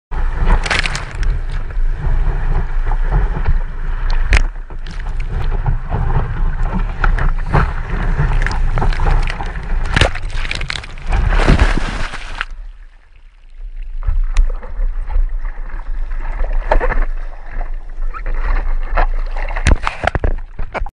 Trace: Surfing - Tomonori Hakko at Unknown beach
Trace auto edited Tomonori Hakko's surfing session at Unknown beach on Jul 18, 2015.

Waves: 6
Min of Surfing: 65.2
Distance: 187 yd
Calories Burned: 343
Max Speed: 14.9 mph